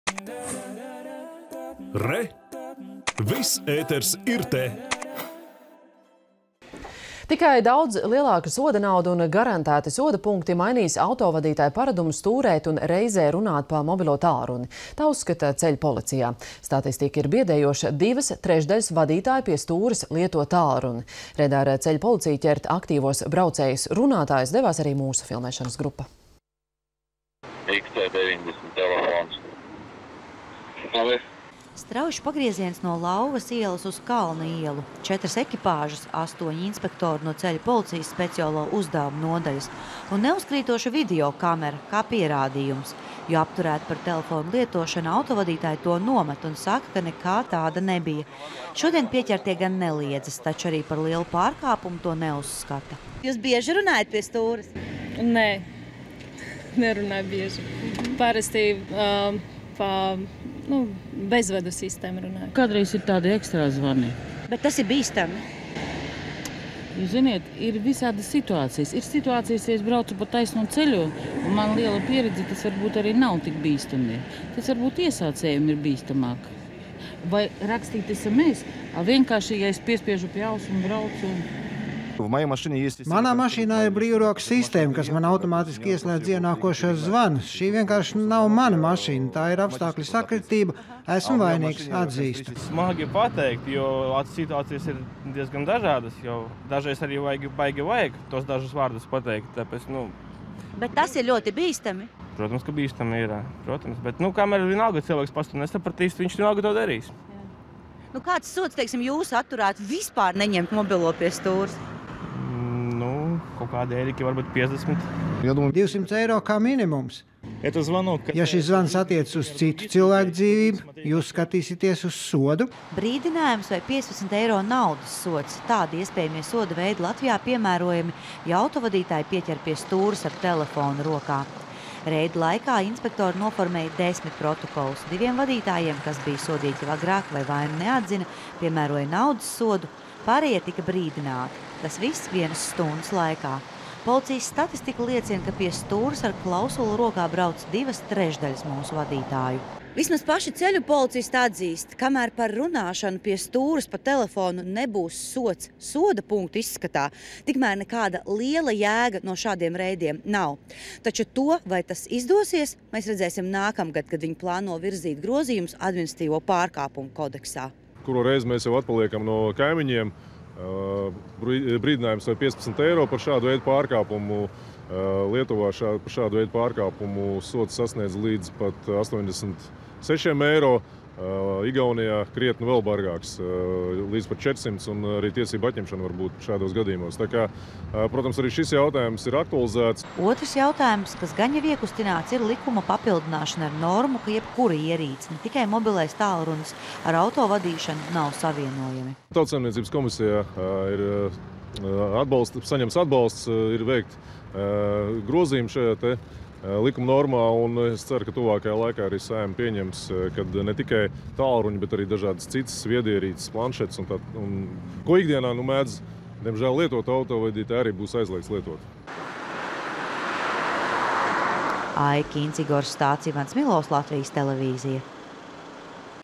2/3 pie stūres runā pa telefonu
Tikai daudz lielāka soda nauda un garantēti soda punkti mainīs autovadītāju paradumu stūrēt un reizē runāt pa mobilo tālruni. Tā uzskata Ceļu policijā. Statistika ir biedējoša – divas trešdaļas vadītāju pie stūres lieto tālruni. Reidā ar Ceļu policiju ķert aktīvos braucējus – runātājus devās arī mūsu filmēšanas grupa.